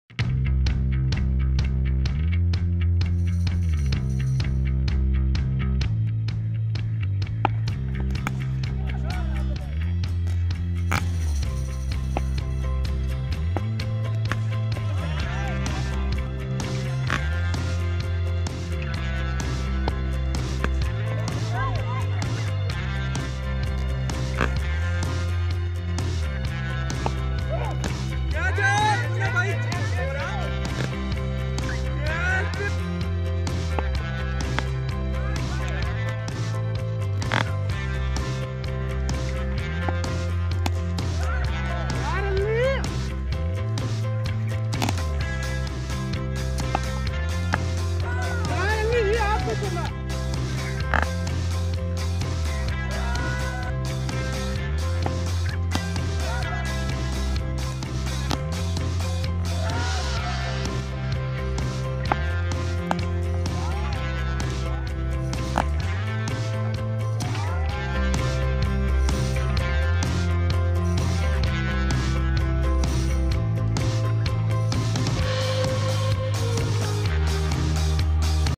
Hafiz hamza ky Sixes | Honey Haroon
Here is new talent from our village. Hafiz Hamza is the most talented cricketer in our village. He is playing very energetically.